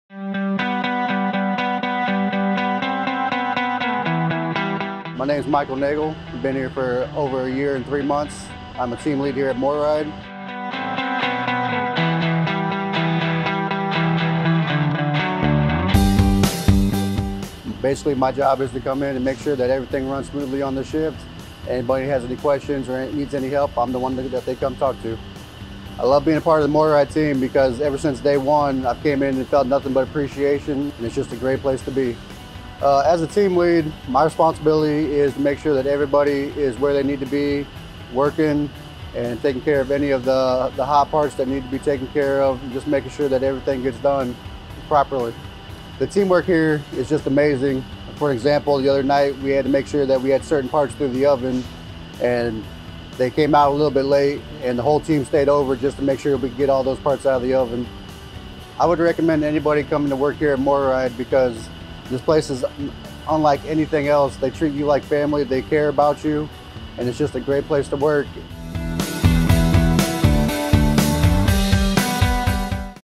Employee Spotlight: Michael Nagle
👥 Team Member Spotlight 👥
Check out this weeks' spotlight featuring Michael!  Take a look as he describes what it's like working at MORryde.  Could you see yourself on our team?  If so, apply today at MORrydeCareers.com